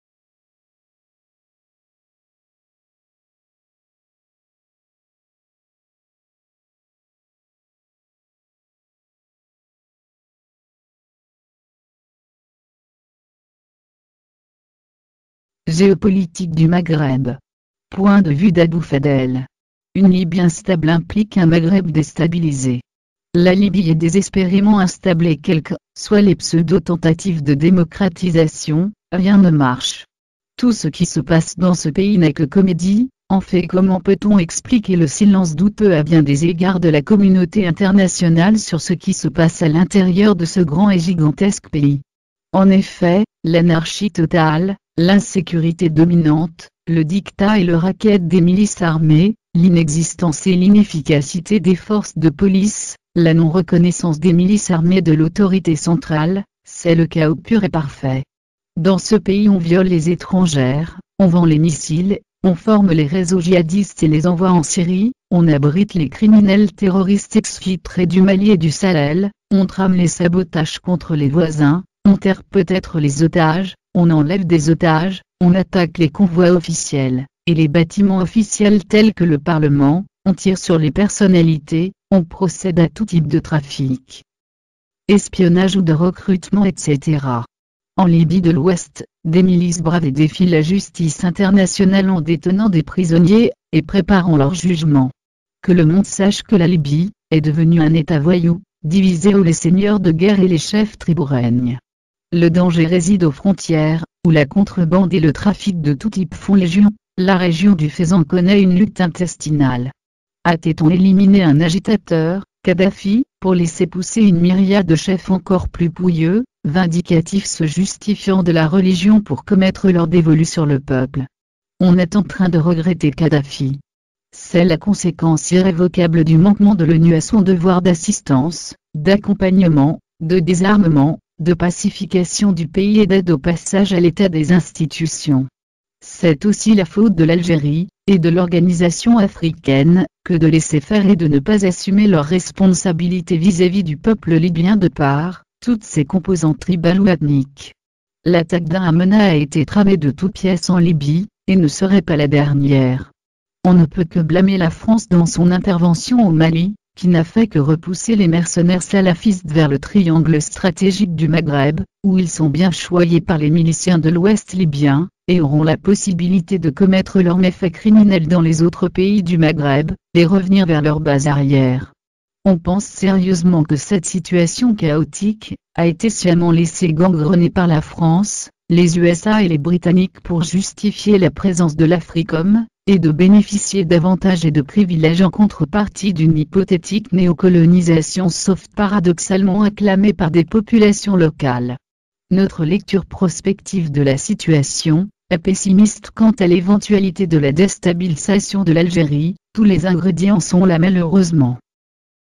Libye instable implique un Maghreb destabilisé
Pourquoi a t-on laissé la Libye sombrer dans le chaos sécuritaire